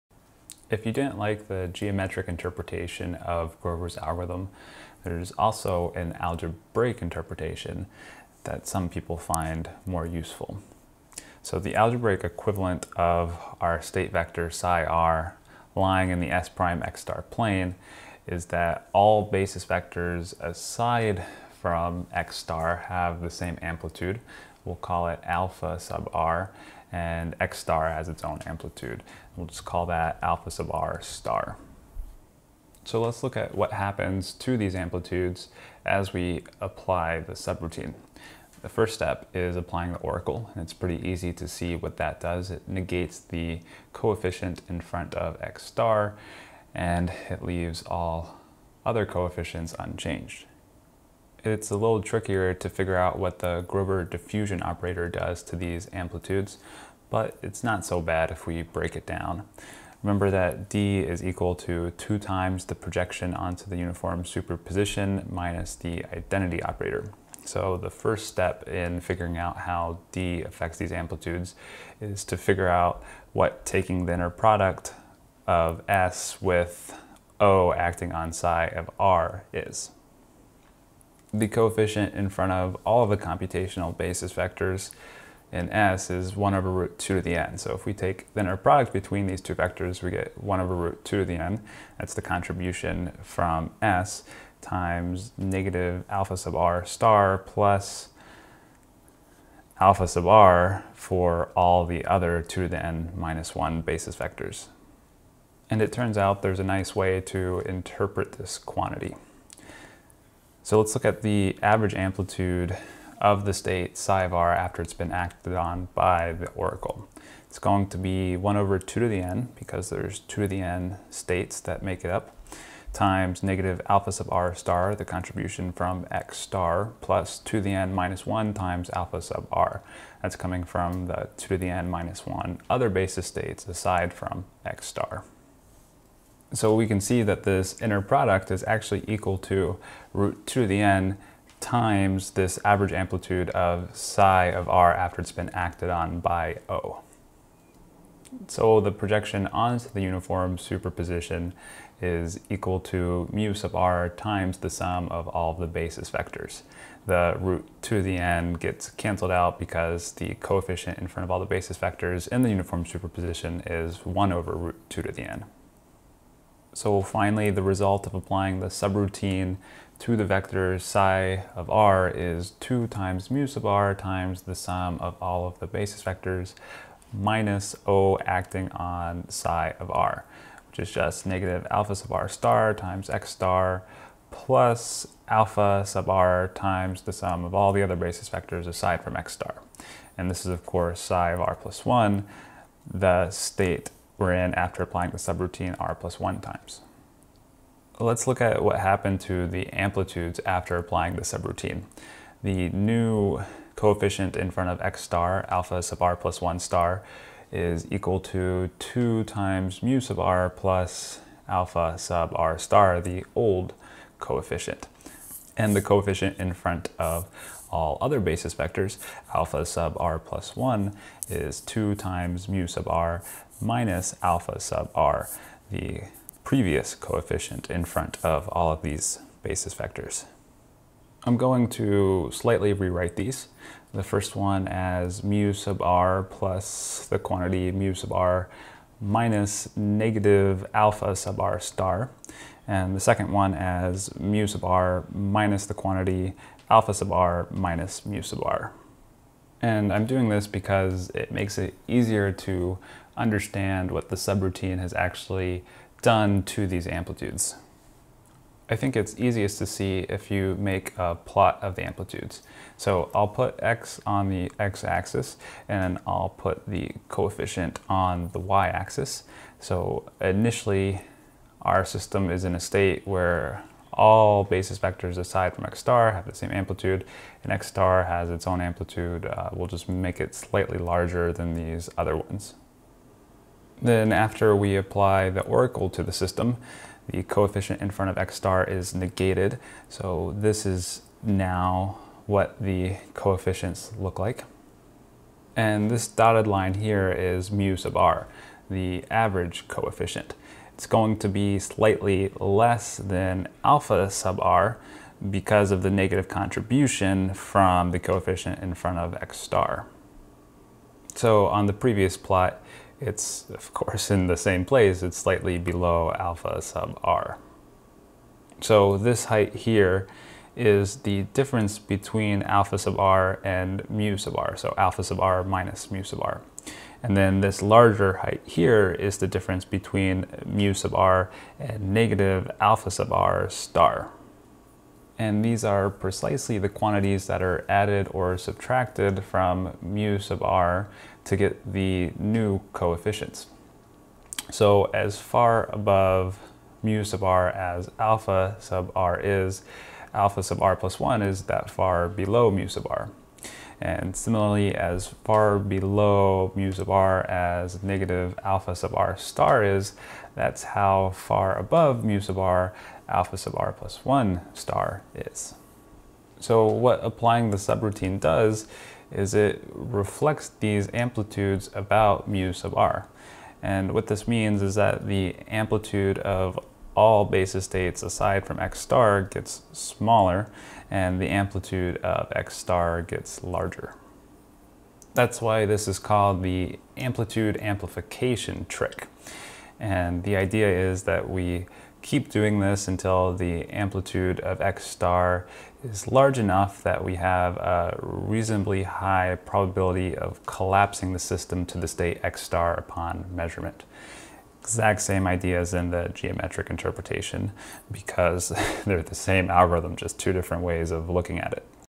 Introduction to Quantum Computing (21.2) - Grover's Algorithm: Algebraic Interpretation
Grover's algorithm can also be understood algebraically. The algebraic equivalent of the state vector lying in the ket(s')-ket(x*) plane is that all basis states in the expansion of the state vector have the same amplitude, alpha_r, except for ket(x*), which has amplitude alpha*_r. O affects the amplitudes in a straight-forward way. D is trickier. The inner product between ket(s) and the state vector after it has been acted on by O is equal to root(2^n) times the average amplitude of the basis states in the expansion of the state vector after it has been acted on by O, mu_r^O. So the result of applying O followed by D is to reflect the amplitudes of all basis states across the average amplitude of the previous state after it has been acted on by O. The result is that the amplitude of ket(x*) gets larger and the amplitudes of all other basis states gets smaller. That's why this is called the amplitude amplification trick.